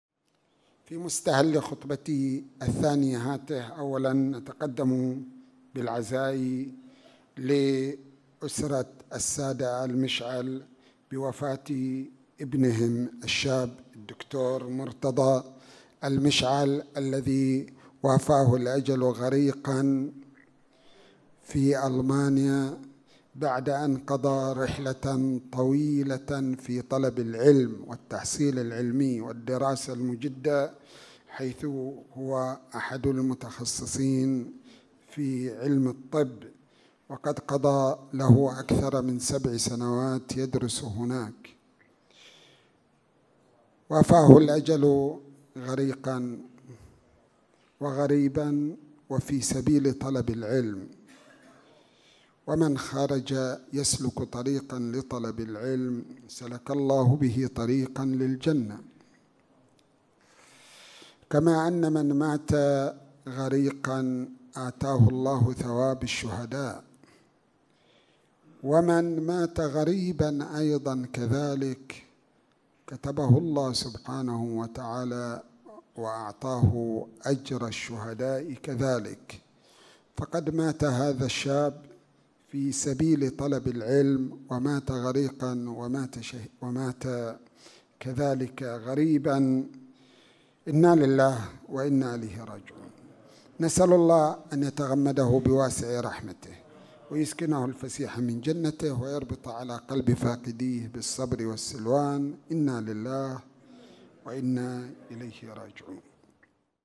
نداء الجمعة || تعزية للسادة آل مشعل
سماحة الشيخ عبدالكريم كاظم الحبيل حفظه الله
ضمن الخطبة الثانية
بمسجد العباس بالربيعة